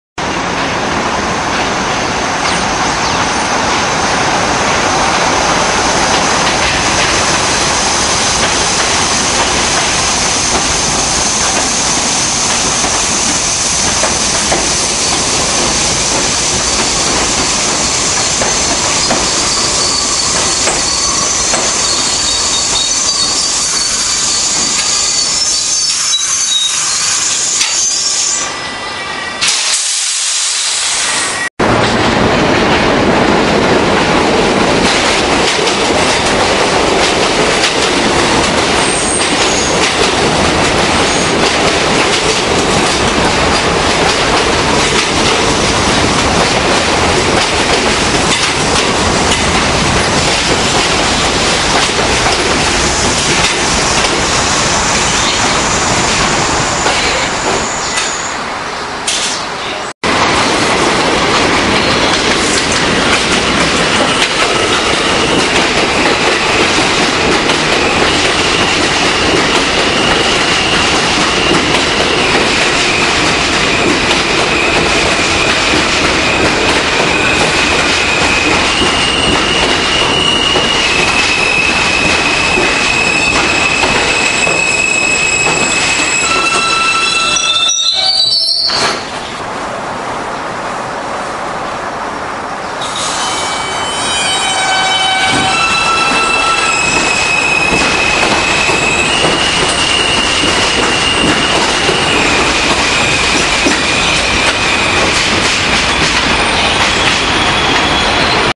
R68/68A (B) & R160B (Q) at Brighton Beach [Chapter 508]
Robbie68 #2860 terminated & Robbie68Amell #5078 (B) arrived & Robbie160Billy #9162 (Q) lcl arrived & departed Brighton Beach.